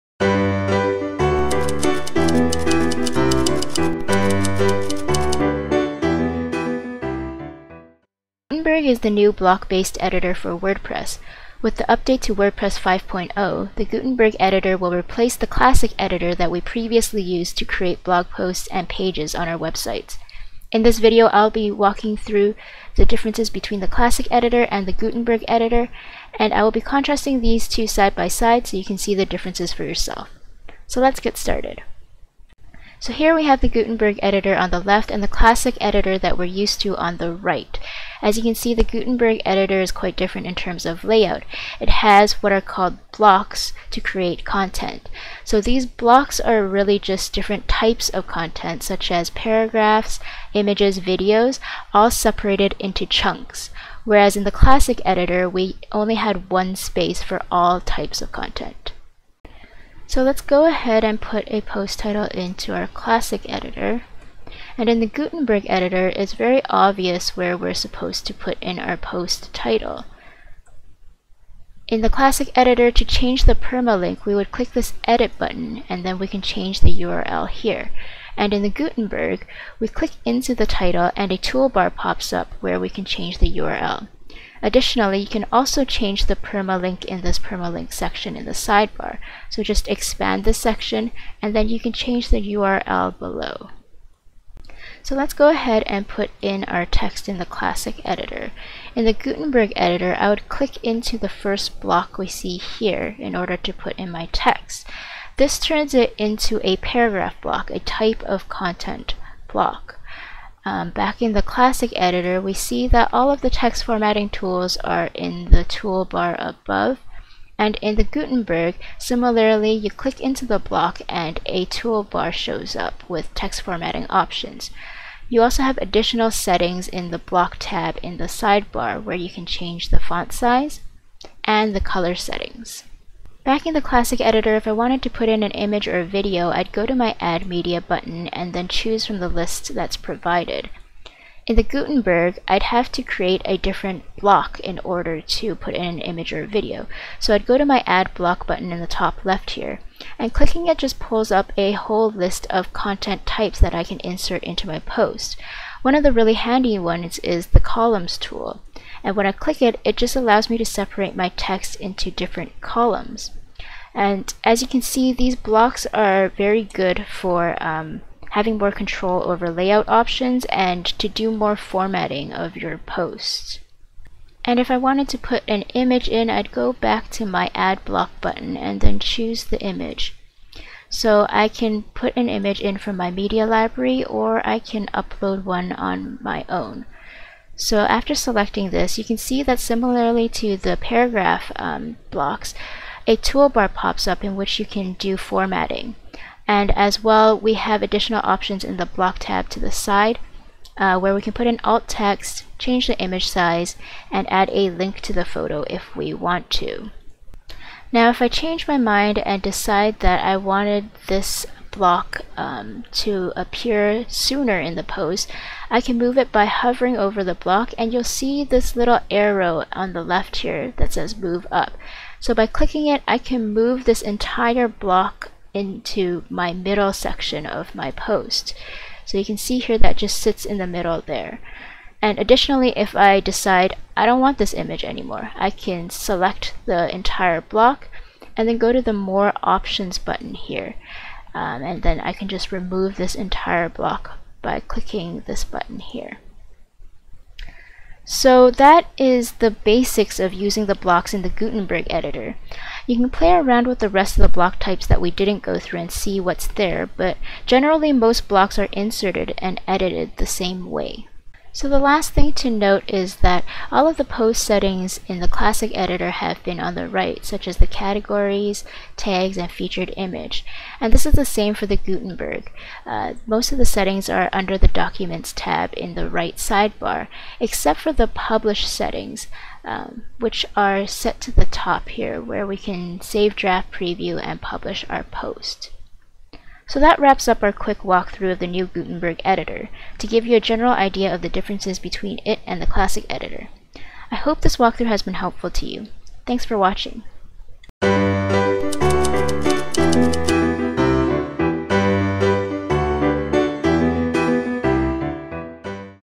Gutenberg Editor Tutorial for Wordpress 5.0
Wordpress launched the new Gutenberg editor to replace their Classic editor in version 5.0. Take a look at how the new Gutenberg editor compares to the Classic editor when you create new pages and posts in Wordpress.

The Gutenberg editor gives you more control over the design and layout of the pages and posts. But if you prefer using the Classic Editor, you'll have to download and install the Classic Editor plugin which can be found